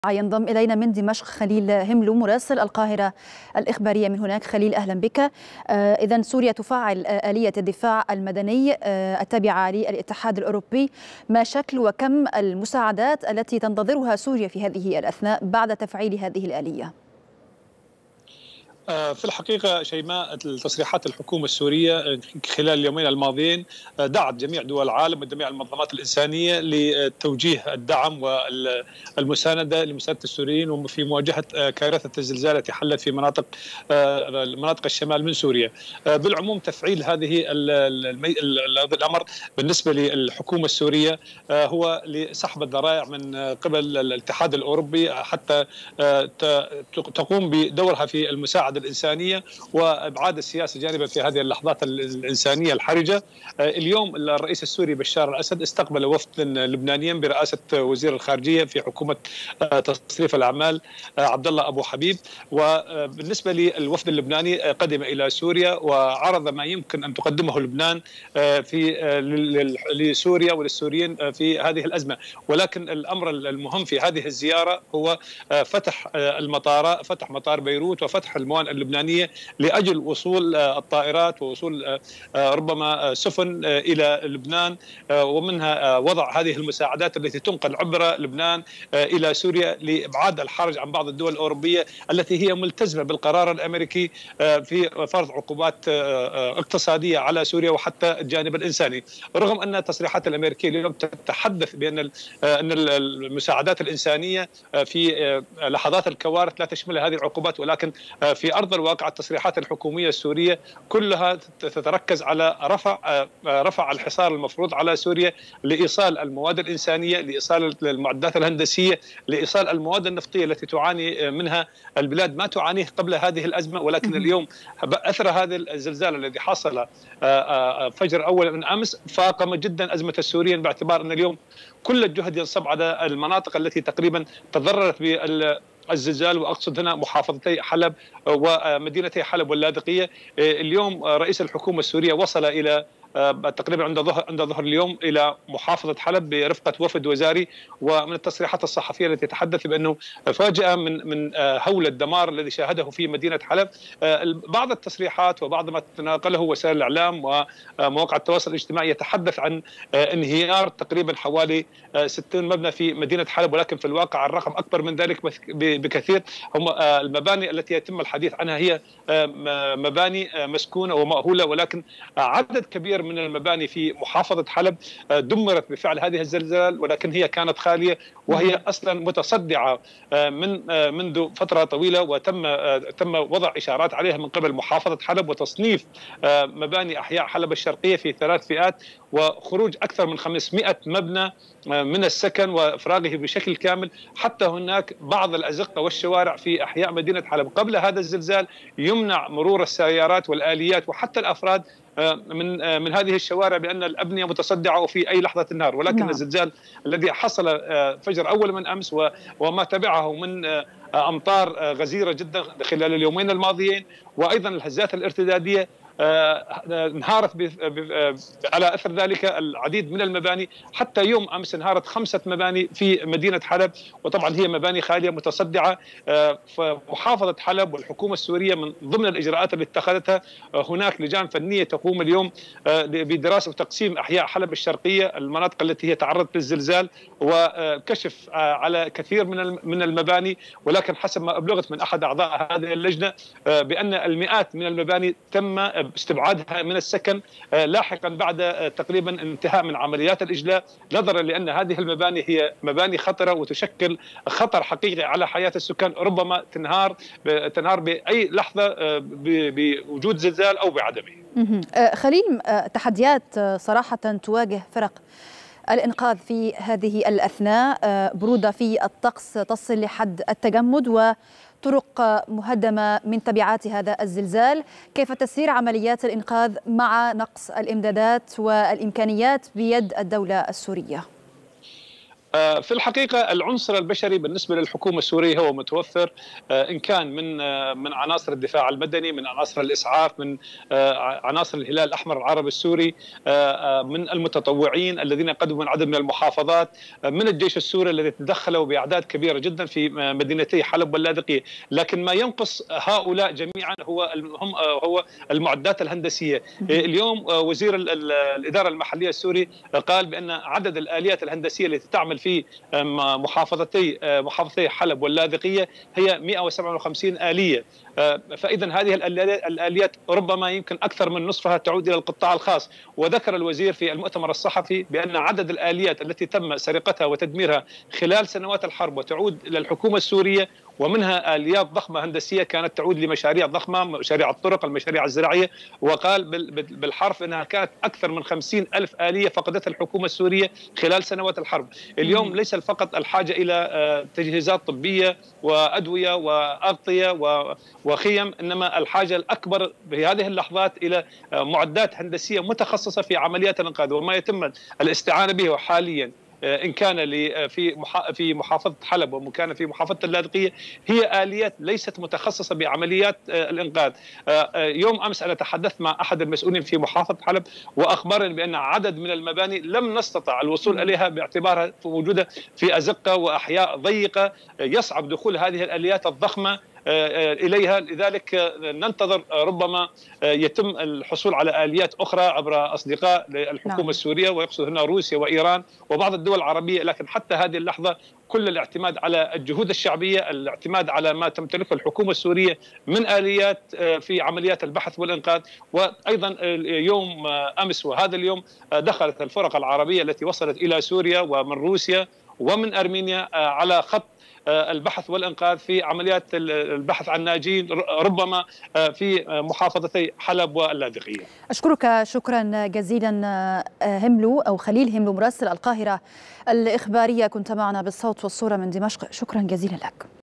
مراسلنا يكشف عن آخر تطورات الزلزال المدمر في سوريا
آخر تطورات الزلزال المدمر في سوريا يكشفها خليل هملو مراسلنا من دمشق

نشرة_الأخبار
8/2/2023